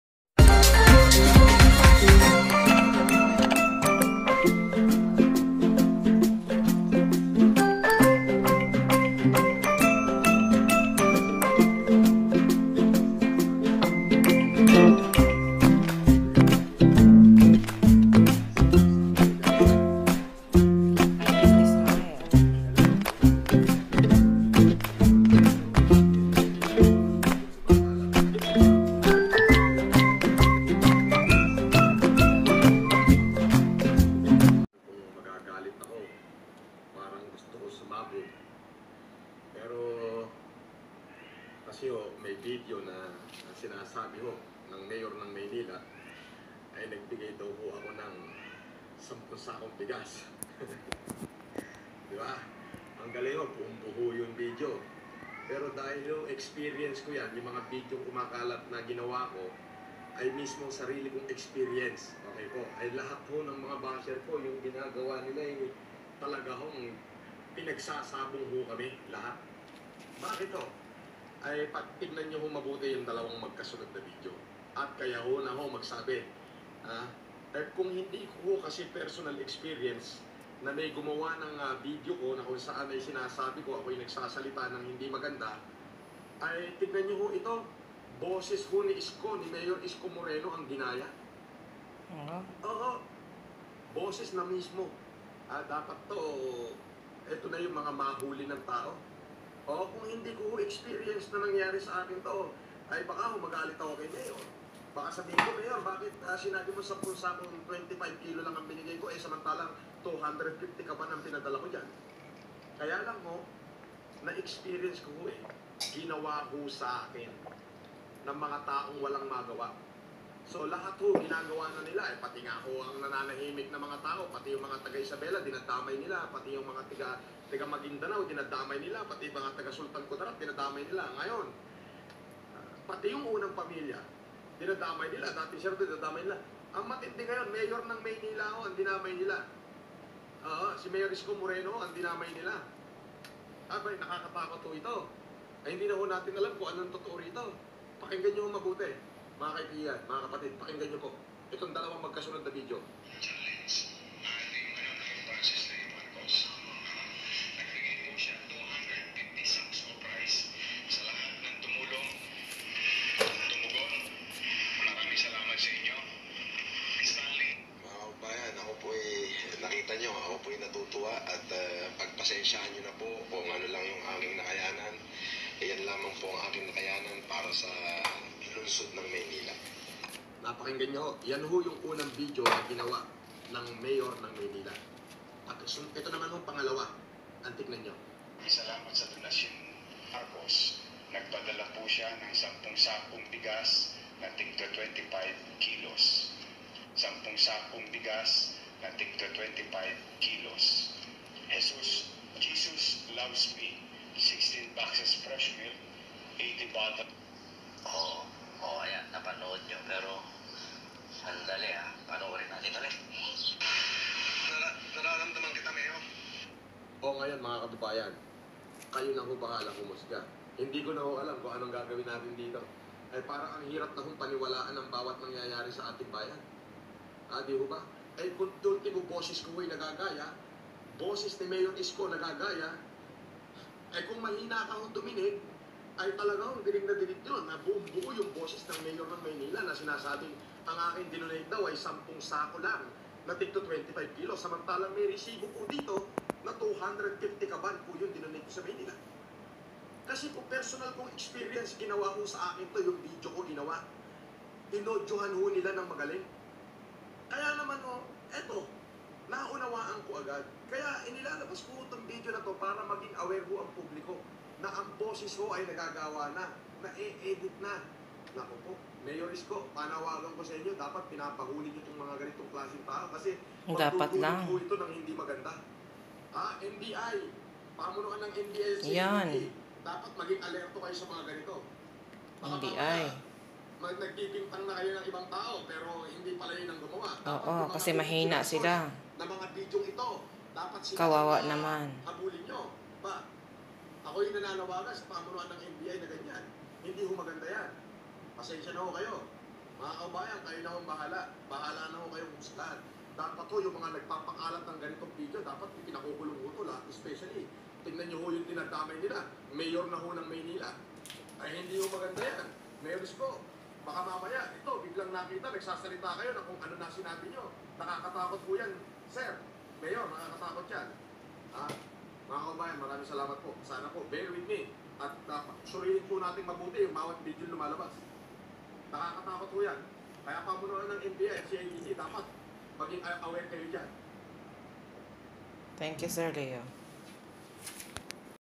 Panawaganan ni Sir FLM sa taga NBI, meron ho bang update???
Anong gagawin mo kung sa iyo ito nagyayari? The truth will prevail. Buti nalang nalaman kaagad before malinlang ang karamihan. Thanks Sir FLM sa iyong pagbubulgar sa panid ng katotohanan. Curious din ako kung ano na ang ginawa sa Taga NBI nito..at .Mayor Isko.